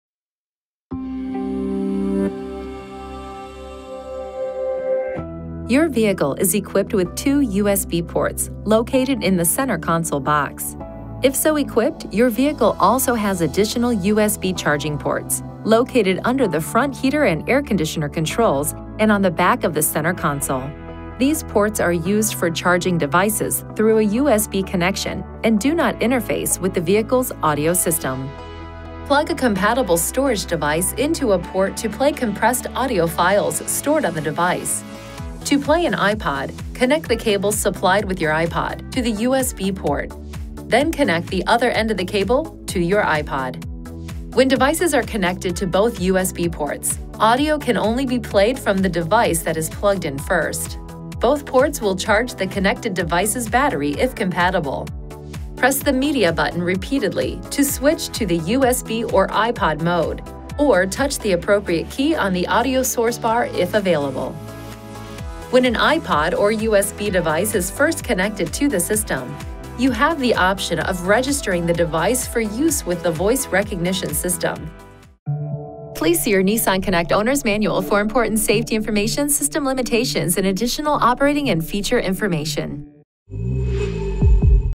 2019 Nissan Pathfinder - USB/iPod® Interface
Your vehicle is equipped with two USB ports, located in the center console box.

If so equipped, your vehicle also has additional USB charging ports, located under the front heater and air conditioner controls and on the back of the center console. These ports are used for charging devices through a USB connection and do not interface with the vehicle's audio system.

Plug a compatible storage device into a port to play compressed audio files stored on the device. To play an iPod®, connect the cable supplied with your iPod® to the USB port, then connect the other end of the cable to your iPod®.

When devices are connected to both USB ports, audio can only be played from the device that is plugged in first. Both ports will charge the connected device’s battery if compatible.

Press the MEDIA button repeatedly to switch to the USB or iPod® mode, or touch the appropriate key on the audio source bar if available.

When an iPod® or USB device is first connected to the system, you have the option of registering the device for use with the Voice Recognition System.

Please see your NissanConnect® Owner’s Manual for important safety information, system limitations, and additional operating and feature information."